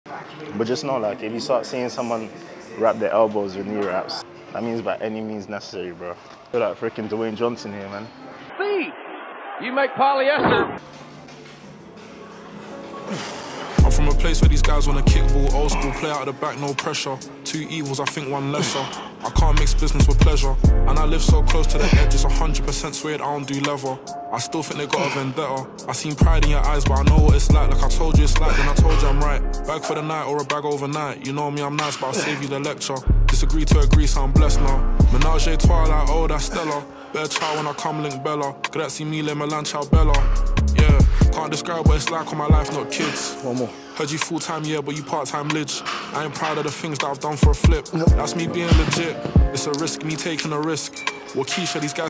KNEE SLEEVES ON YOUR ELBOWS?
I was looking for a pair of elbow sleeves on amazon and i saw a picture of what looks like knee sleeves around someone's elbow. BET. We are moving weight now like never before.